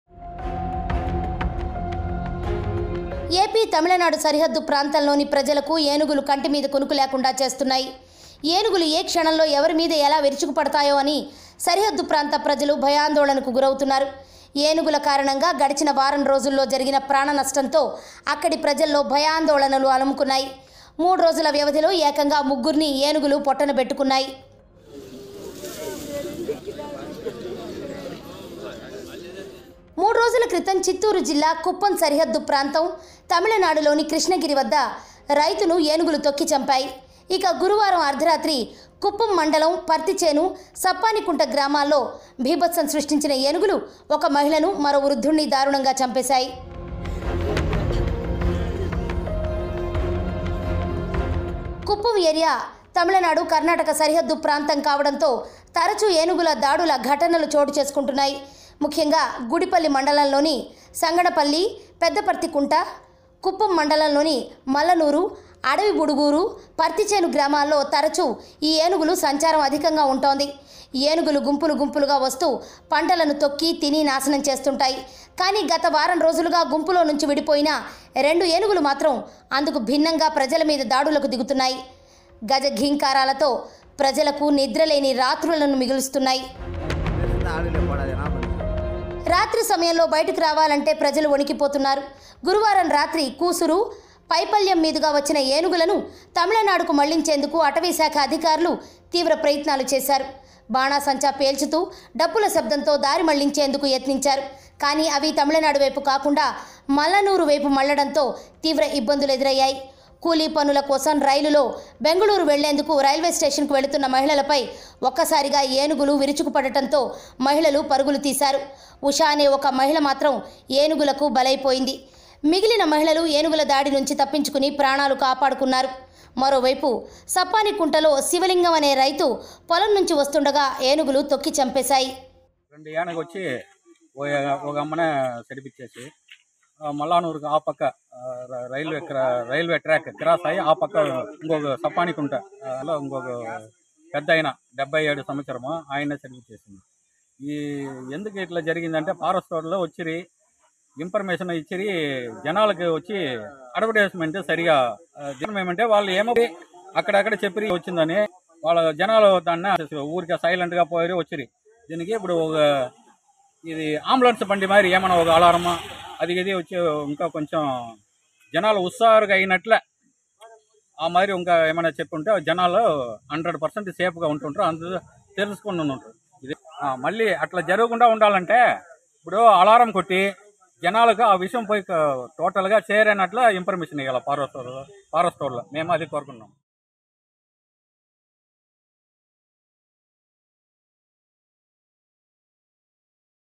A Woman Died in Elephant Attack near Kuppam | Chittoor District | Samayam Telugu
Tragedy took place in Kuppam constituency. A woman died in an elephant attack in Mallanur panchayat. Usha, who hails from Partichenu village, commutes to work from Mallanur to Bangalore by train during the day. As usual she left the village along with two other women to board the train. But on the way to the railway station, other two women escaped with injuries when they were attacked by elephants.  Usha died in an elephant attack. Usha has three children and her husband is dead. Revenue and forest department officials have reached the spot and are investigating.